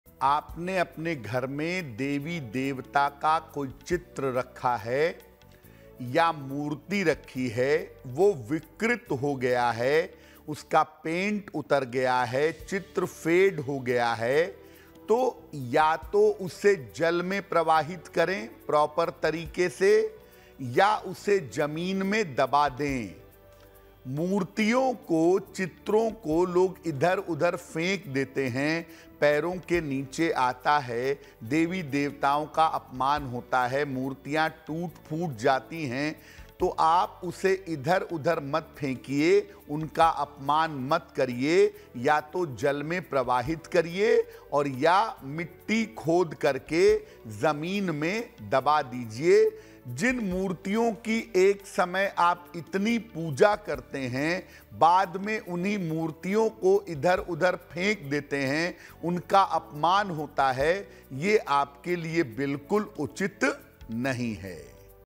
Good Luck Mantra: देवी-देवताओं की ऐसी मूर्तियों से घर में होता है नुकसान
इस वीडियो में पंडित शैलेंद्र पांडेय 18 अप्रैल 2024, गुरुवार का गुडलक मंत्र बता रहे हैं. अगर देवी-देवता का चित्र या मूर्ति विकृत हो गयी हो तो उसे जल प्रवाहित करें या जमीन में दबा दें. इसे इधर उधर बिलकुल न फेंके.